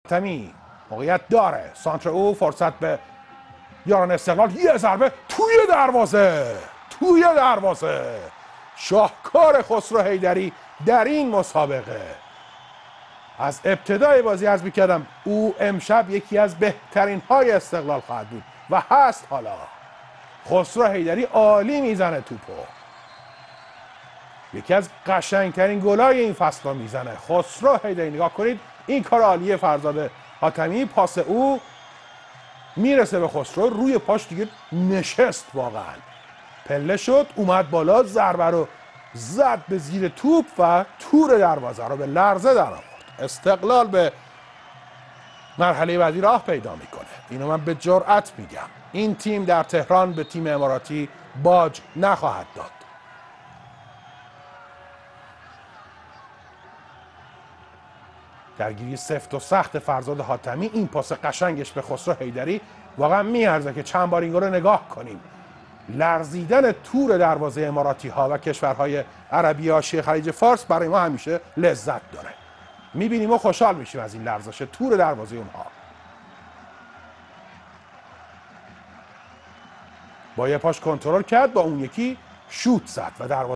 AFC Champions League 2013 * Gool 15 * Round 1/8 * Khosro Heidari * AlShabab UAE_Esteghlal
ليگ قهرمانان آسيا
فصل 2013 -- مرحله یک هشتم نهایی (رفت)
استادیوم مکتوم بن راشد دبی امارات
الشباب امارات -- استقلال ايران
خسرو حیدری